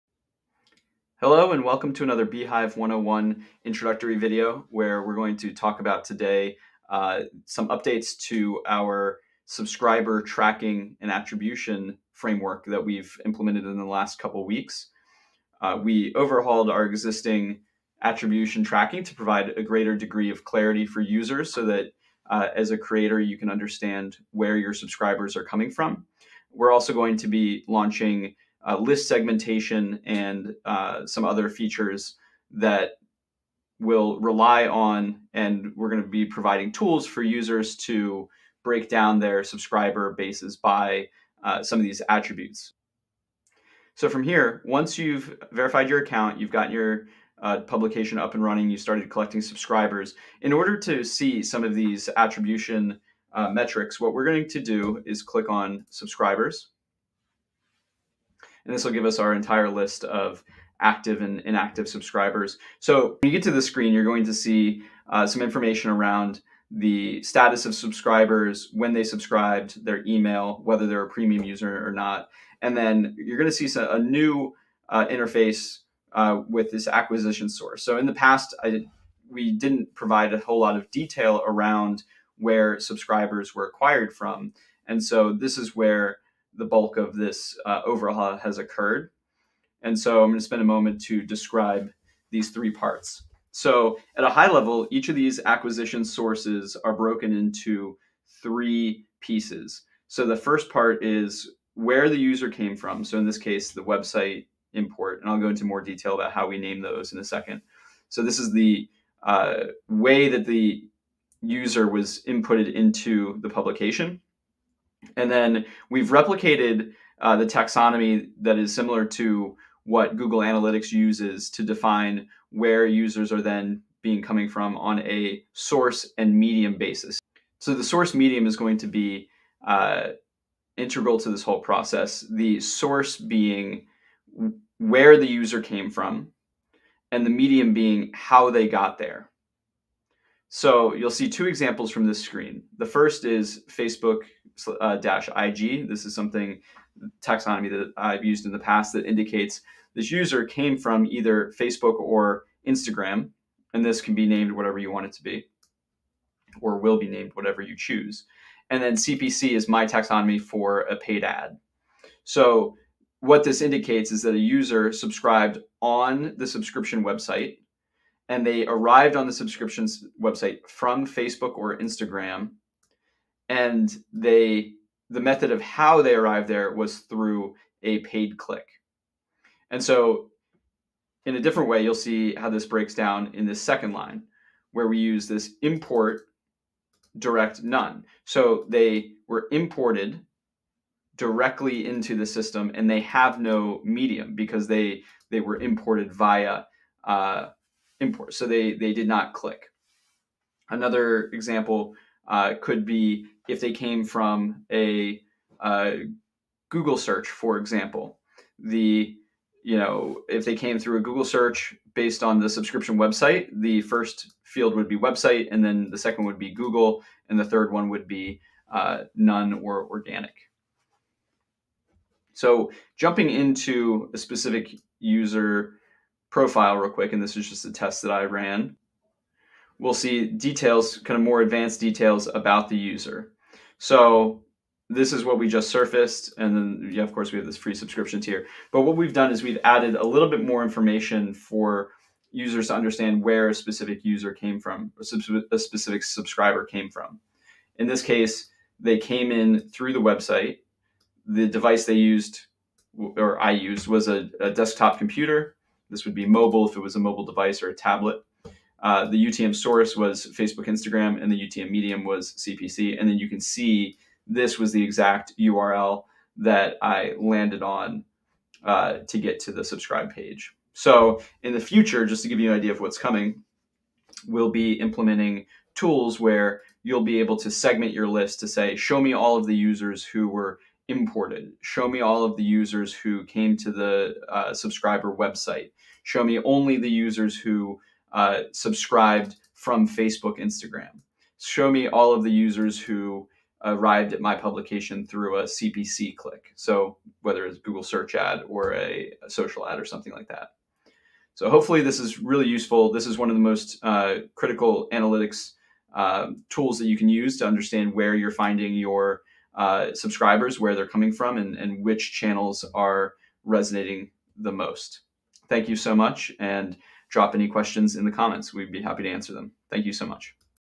Introduction to subscriber attribution - beehiiv 101 (Tutorial)
In this video, we cover beehiiv's subscriber attribution tracking capabilities and how to understand where are your subscribers are coming from and how they're finding you on the web.